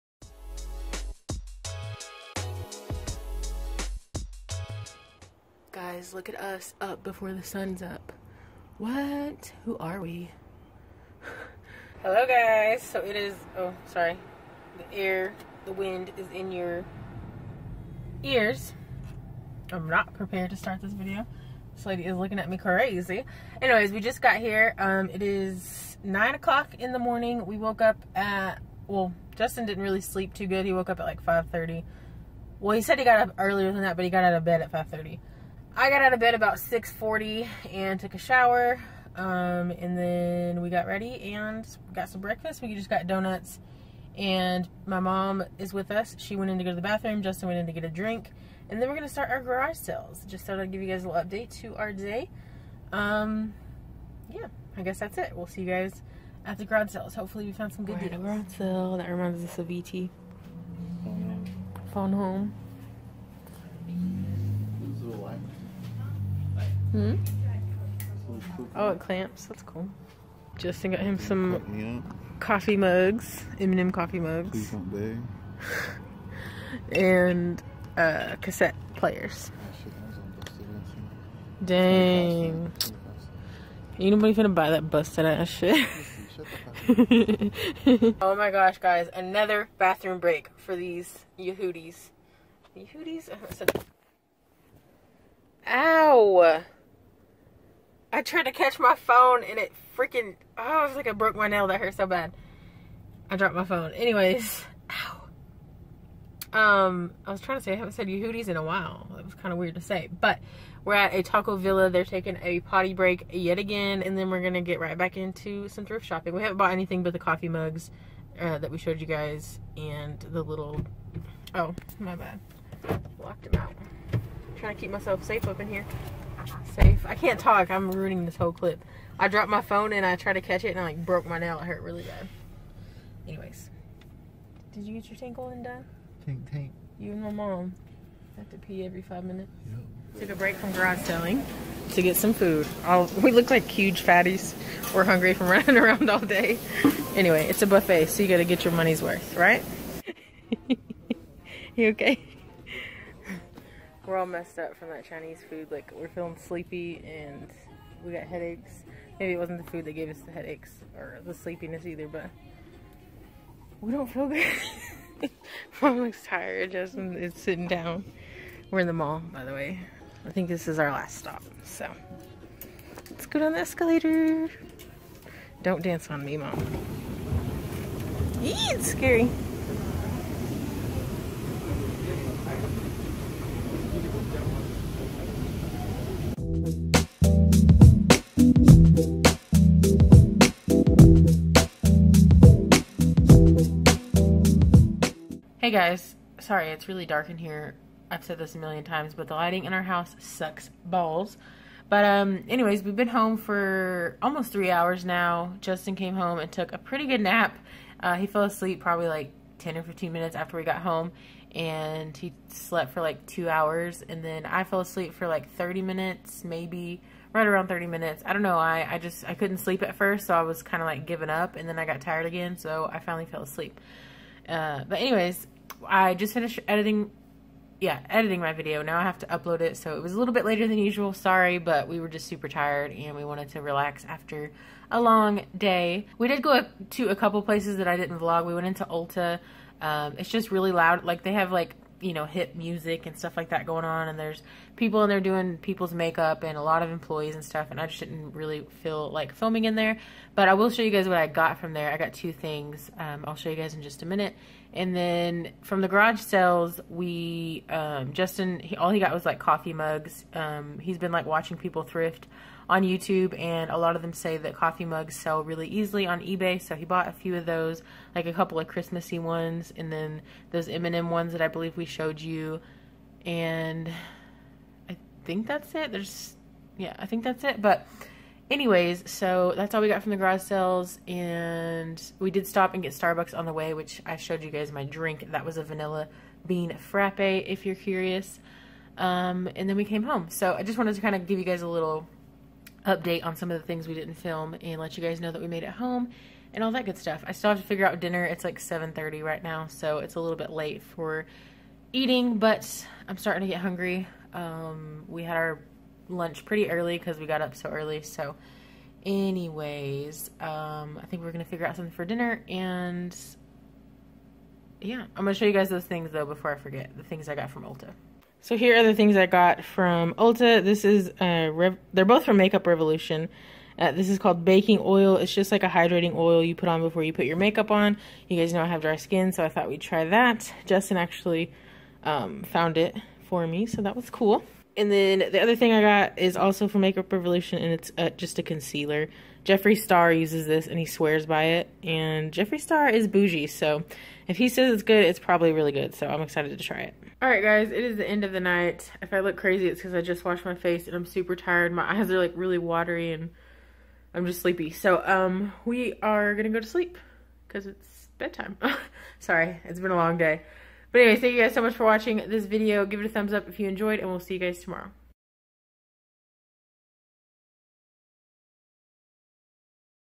Our Garage Sale Finds!
Today was a fun day of shopping all day! We found some good garage sale items and we hope to sell them for a profit!